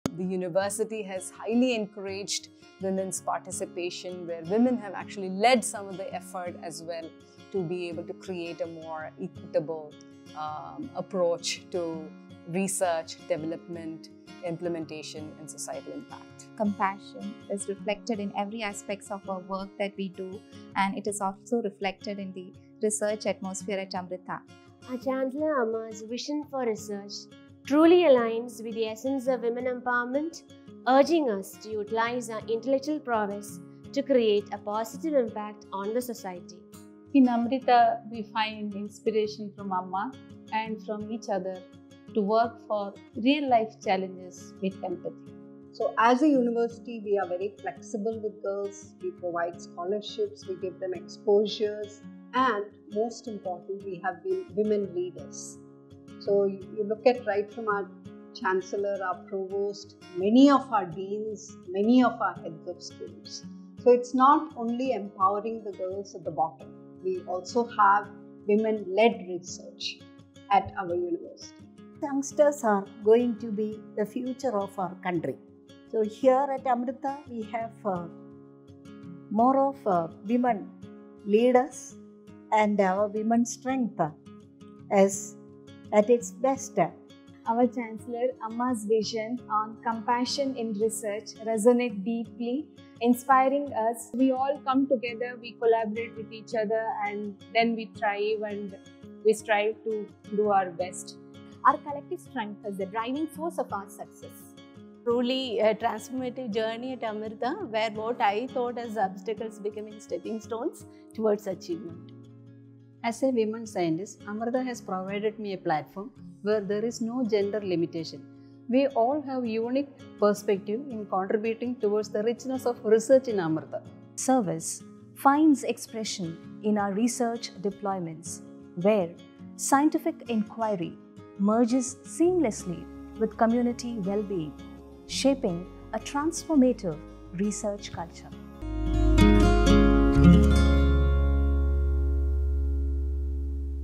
Empowering Innovation: Amrita Research Faculty on Pursuing Excellence and Celebrating Women's Day
Join us on a special Women's Day tribute as Amrita's brilliant women scientists share insights into their research ecosystem and the empowering support they receive. Discover inspiring stories and celebrate the achievements of these remarkable researchers shaping the future at Amrita.